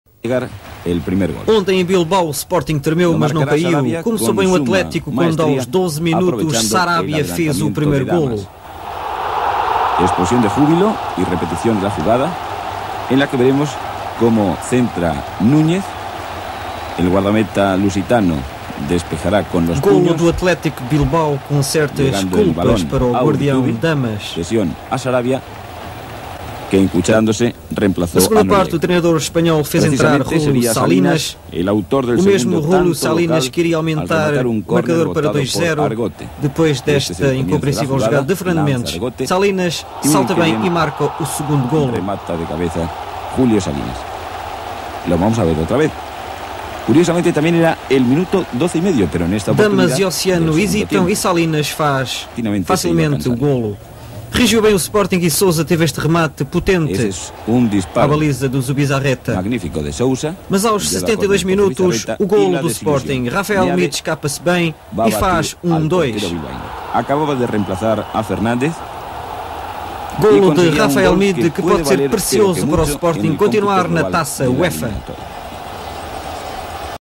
Atl. Bilbau - 2 x Sporting - 1 de 1985/1986
Golos: Sarabia e Julio Salinas (Bilbau) e Raphael Meade (Sporting)